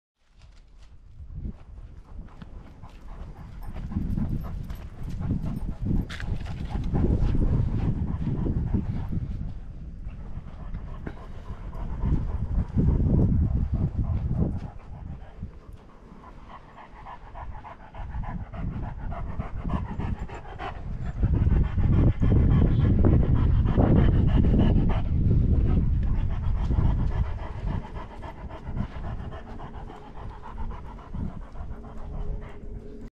Gran danés Gala en el bosque de la primavera
"¡Mira a Gala, la imponente Gran Danés en el hermoso bosque de primavera! Este gigante amigable se divierte mientras explora su entorno natural. ¡Prepárate para verlo saltar y jugar mientras disfruta del aire fresco y los rayos de sol! ¡No te pierdas la oportunidad de ver a este gran amigo en acción!"